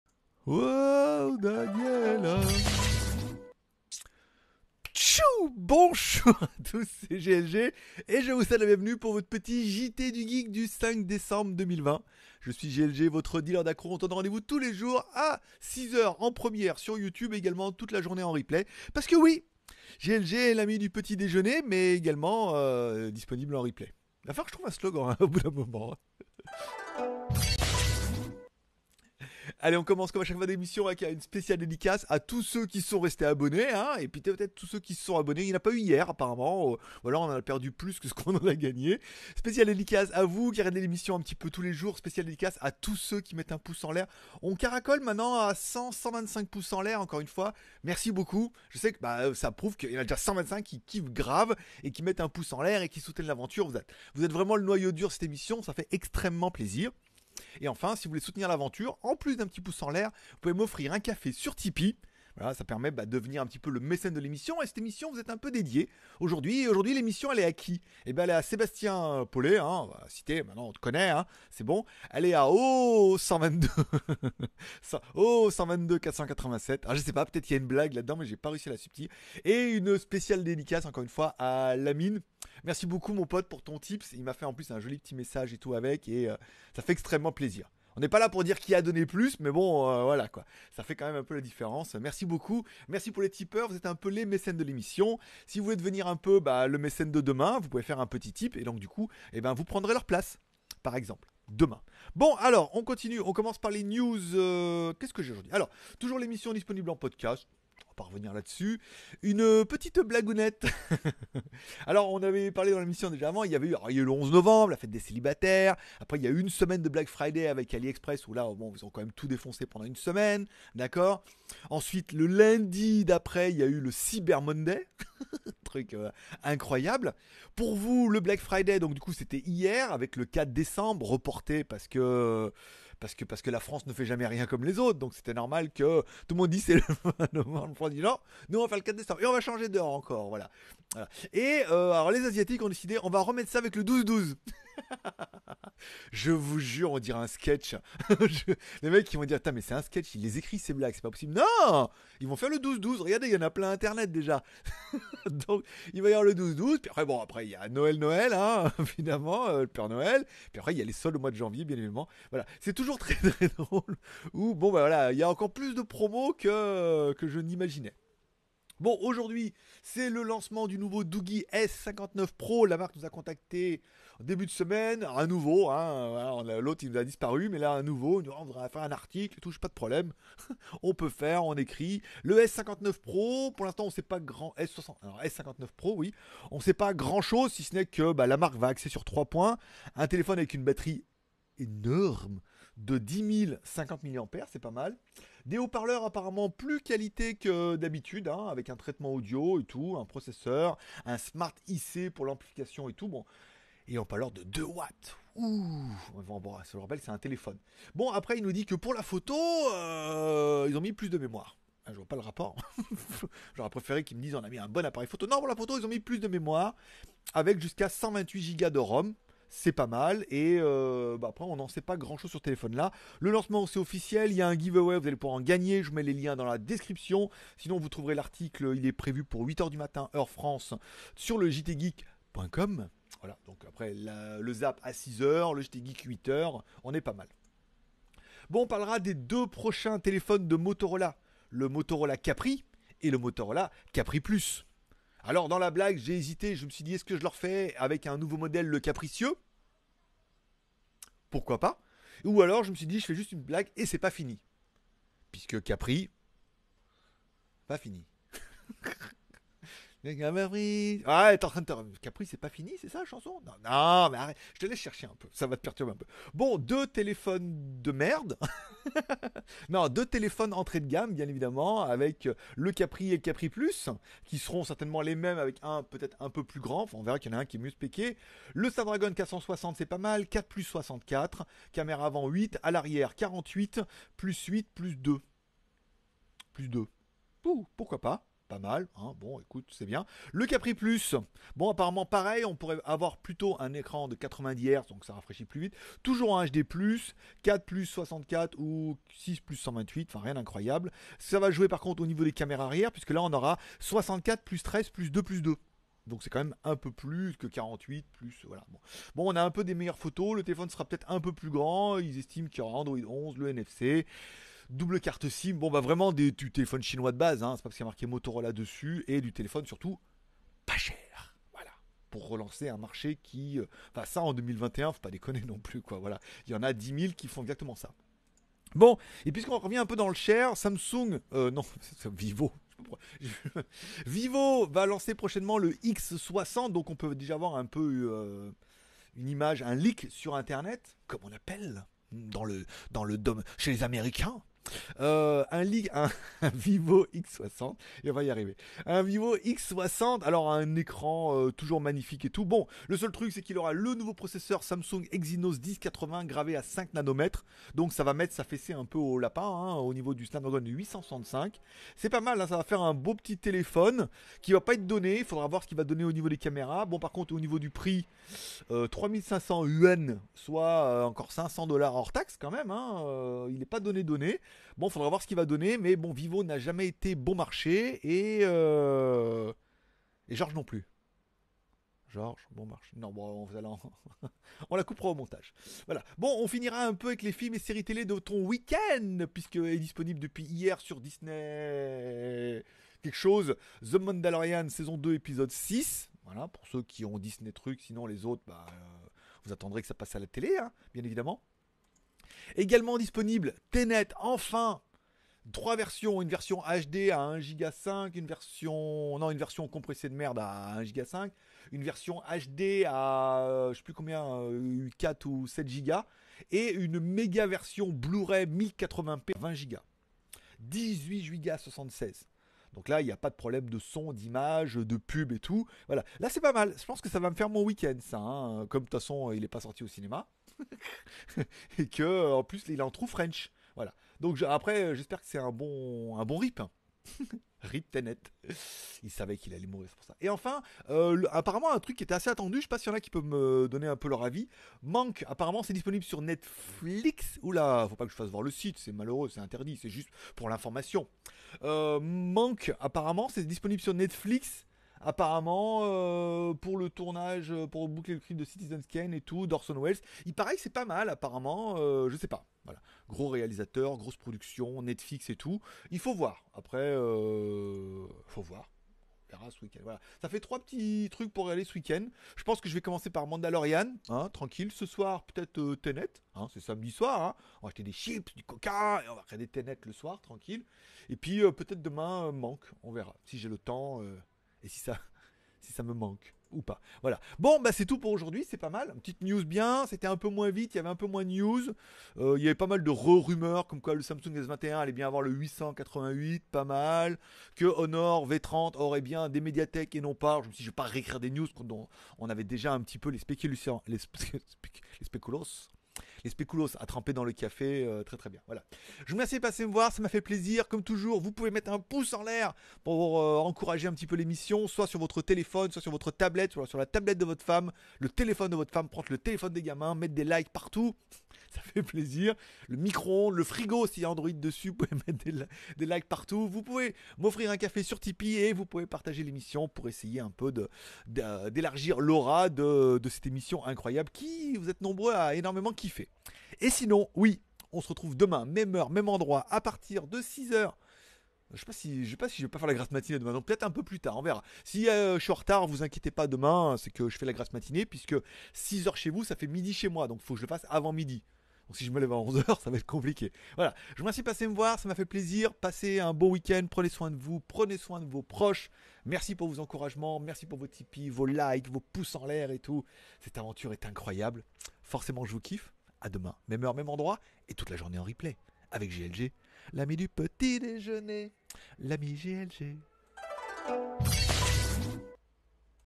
JT DU GEEK 5 dec avec DOOGEE S59 PRO, Motorola Capri, Vivo X60 et 12.12
Le Zap actu High-Tech by GLG , un petit résumé en Vidéos des dernières news tech et smartphone du Jtgeek @GLG reviews
Doogee S59 pro

DOOGEE S59 PRO, Motorola Capri , Motorola Capri Plus, Vivo X60

By GLG